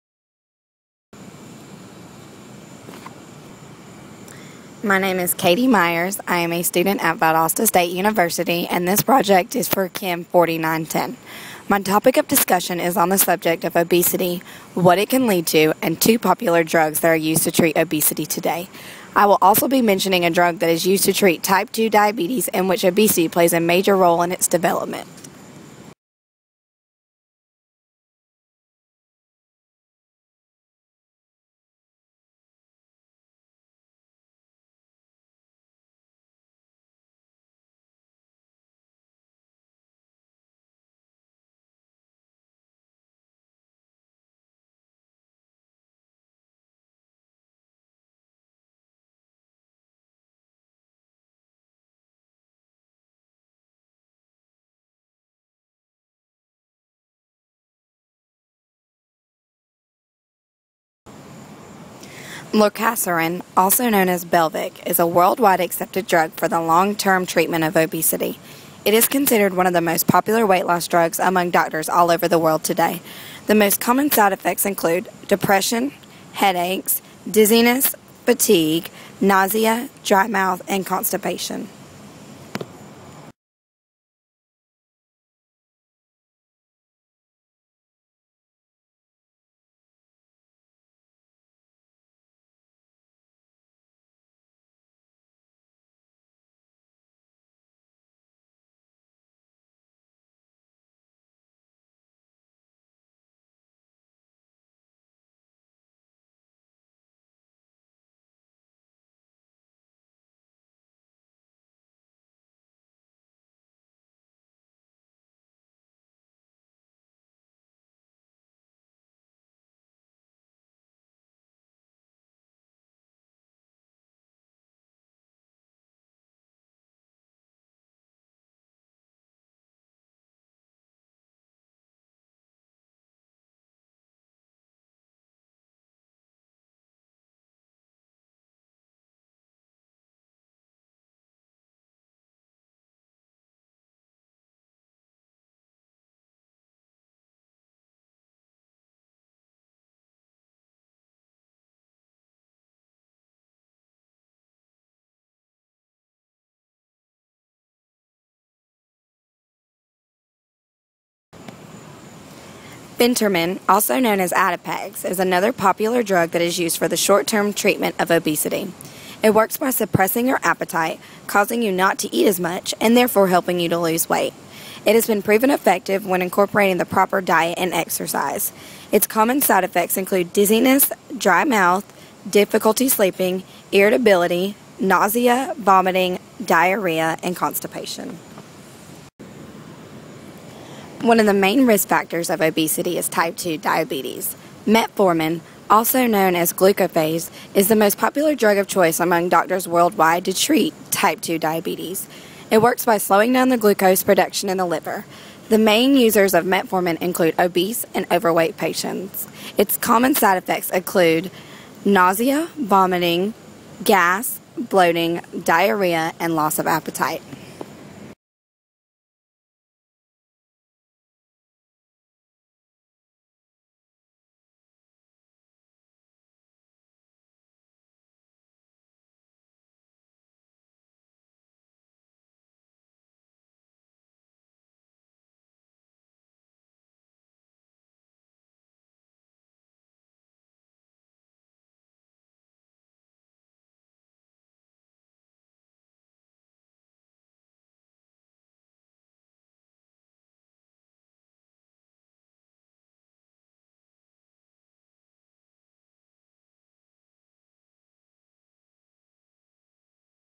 Obesity drug treatment
just a quick and to the to the point video of 2 drugs that treat obesity and 1 drug that treats type 2 diabetes. hope it helps someone out there :)